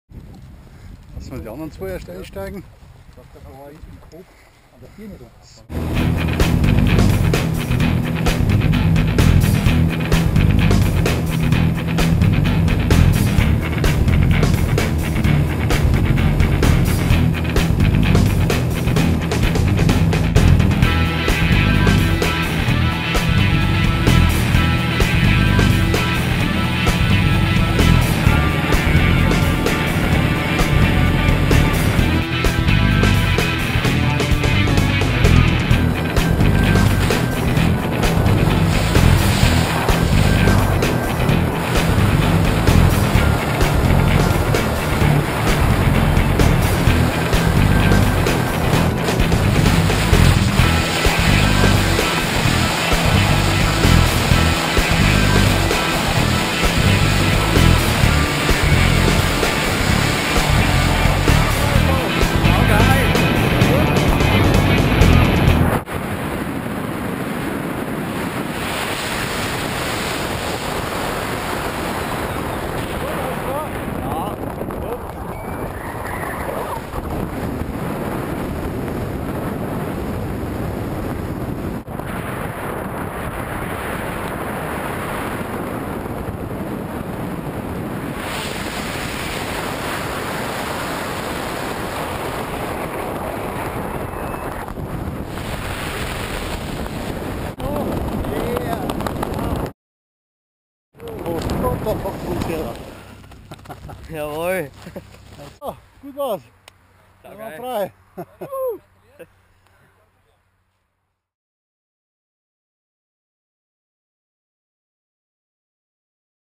Tandemspringer David in Skydive Dingolfing Niederbayern nähe Landshut
David kam zum Fallschirmspringen mit nach Dingolfing Niederbayern zwischen Landshut und Deggendorf. Der Flugplatz ist gut zu erreichen von München, Regensburg, Straubing, Bayerischer Wald usw.